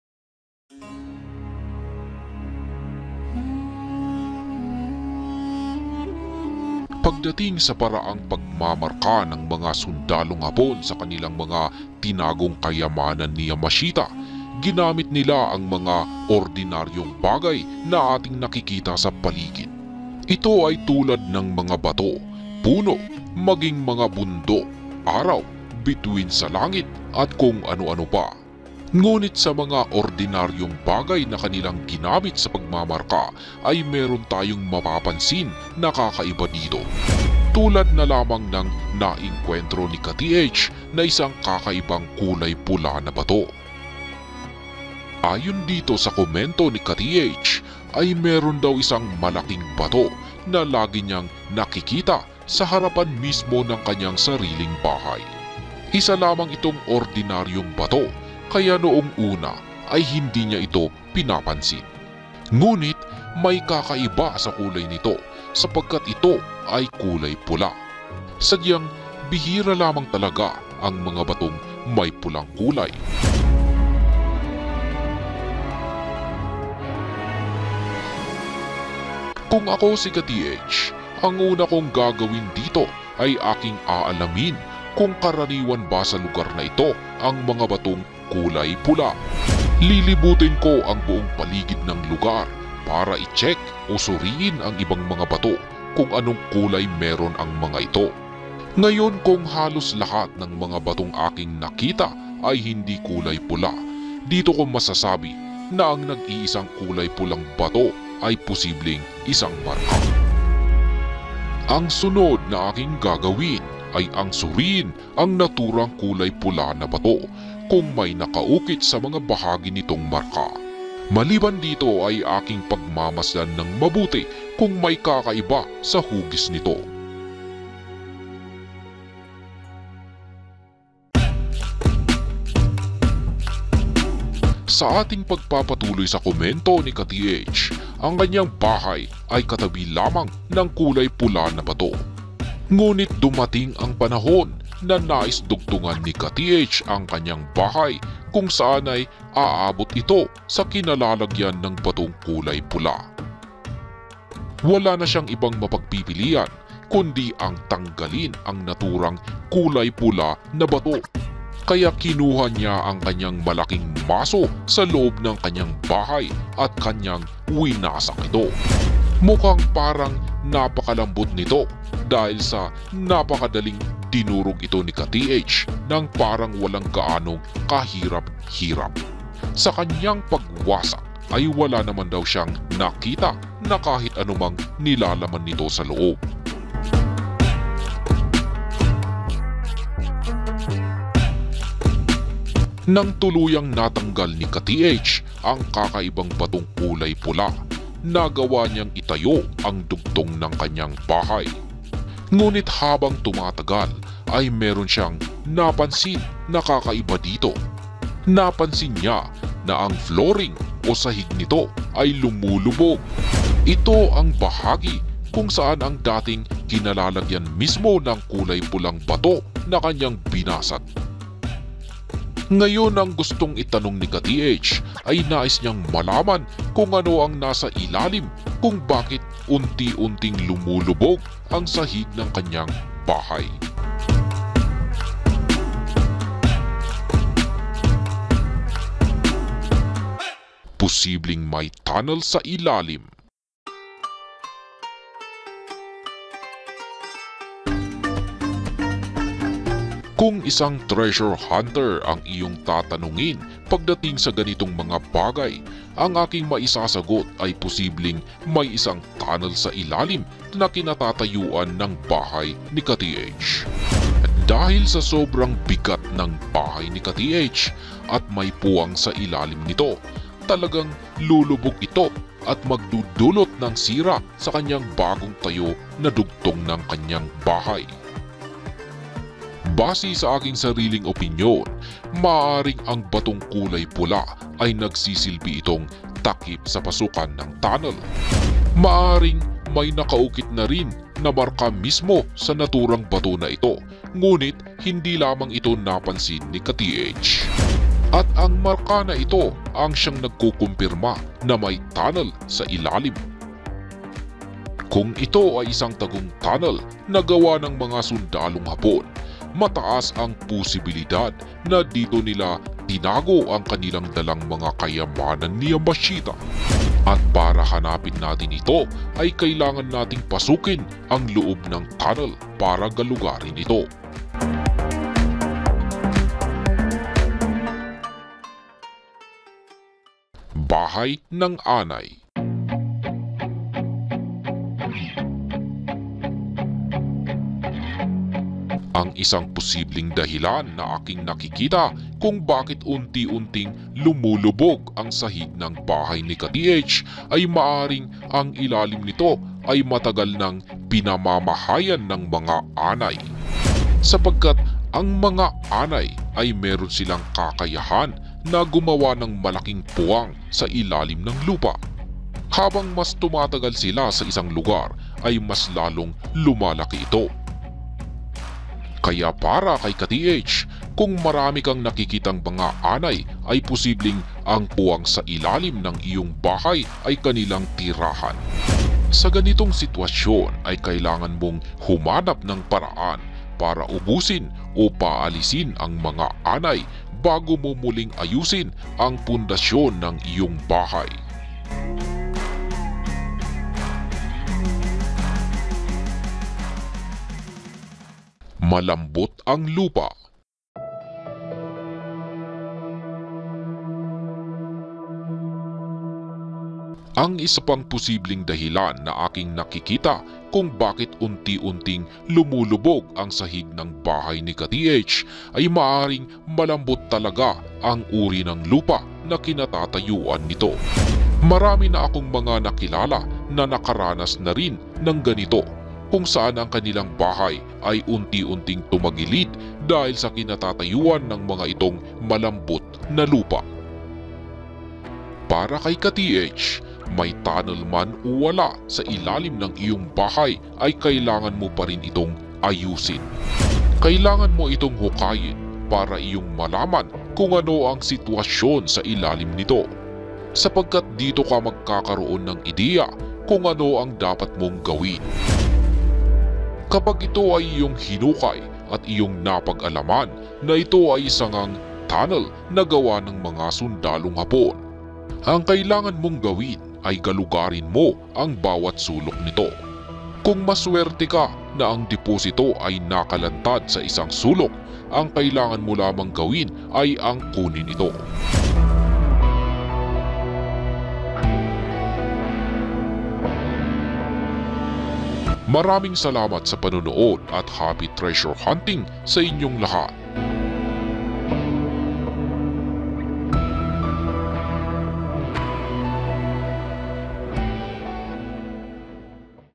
LUMUBOG ANG BAHAY NI KA TH | KULAY PULA NA KAKAIBANG BATO | RED ROCK | TUNNEL UNDERGROUND
Ayon kay ka-TH ay bigla na lamang daw lumulubog ang sahig ng kanilang bahay. Kaya ang kanyang tanong ay kung may tunnel kaya sa ilalim nito.